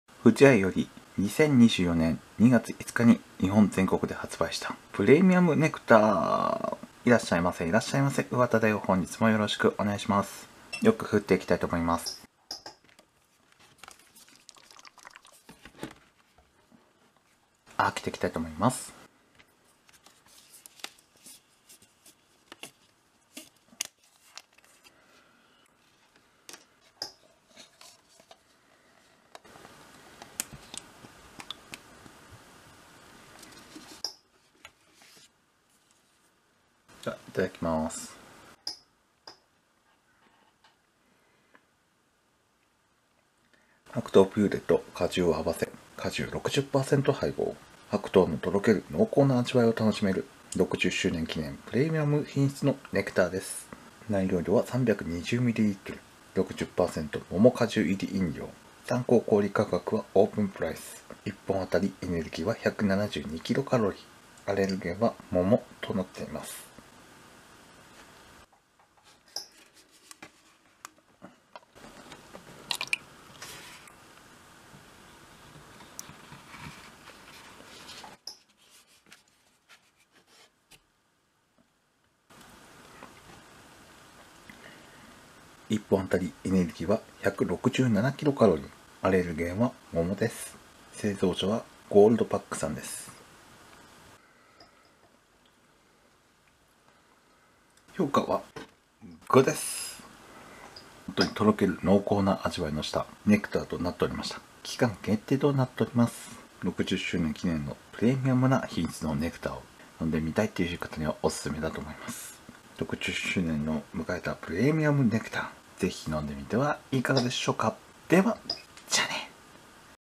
【不二家】プレミアムネクターを飲んだよ！【ネクター】 発売60周年！桃の濃厚さととろける美味しさが味わえるネクター！【伊藤園】
いらっしゃいませ、いらっしゃいませ、んわただよ！
YouTubeで20時以降に動画を出しています。新商品の商品紹介動画を中心に動画出してます。